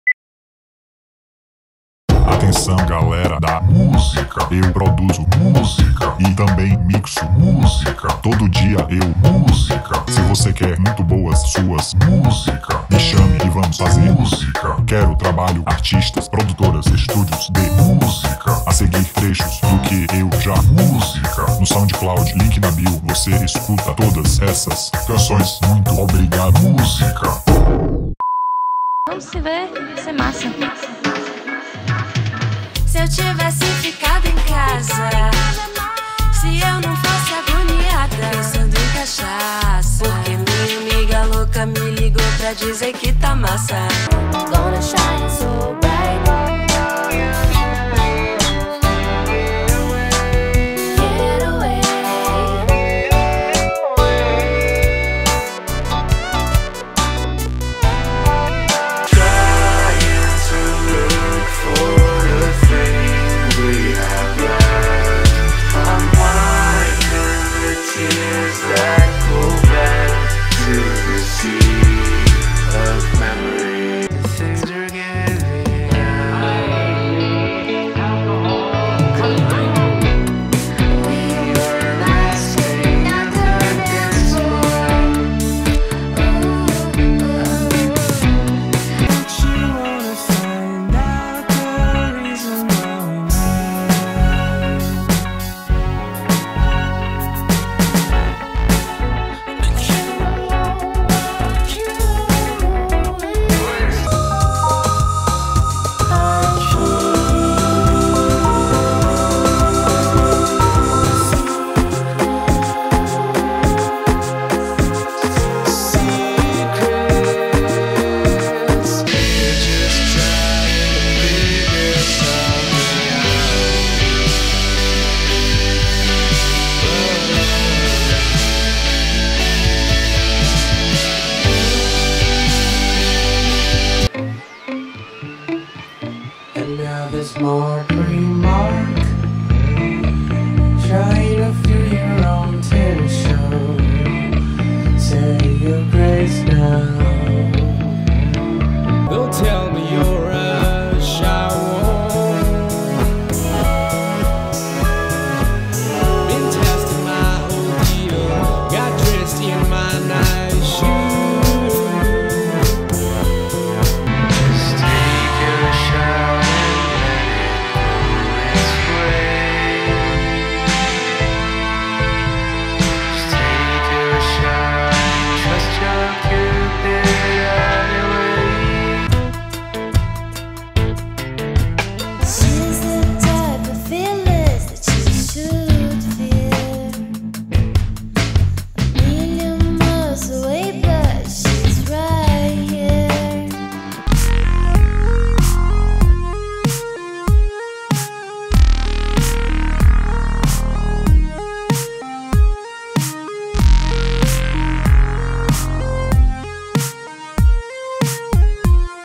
Diego Almeida music production & mix reel 2022
oi internets eu trabalho com produção musical e mixagem
ssa/sp/wrvr/wtv
bookings infos chama:
digiego [at] gmail
vlw bjs